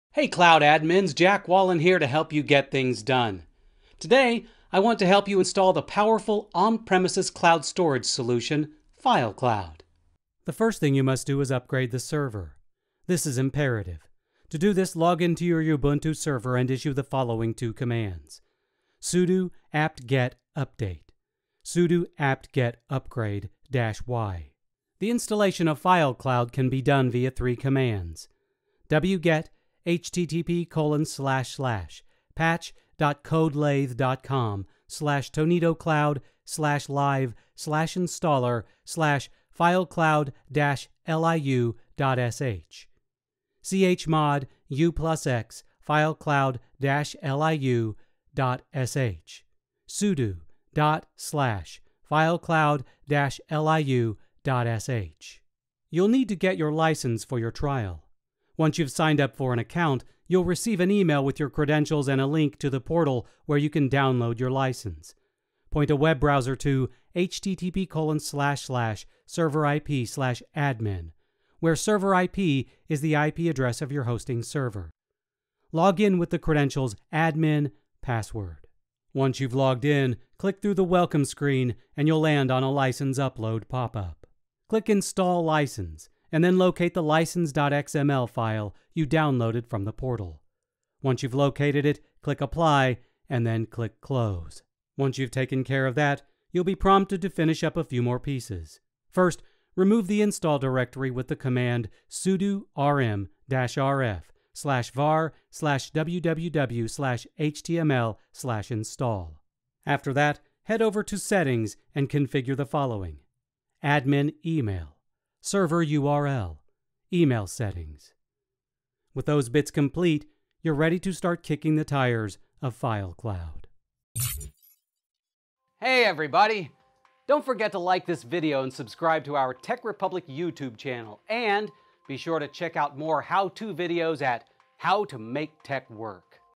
How to install the FileCloud server on Ubuntu 16.04
If you're in need of an on-premises cloud server solution, FileCloud might be what your business is looking for.

List of Commands:

chmod u+x filecloud-liu.sh
sudo ./filecloud-liu.sh
sudo rm -rf /var/www/html/install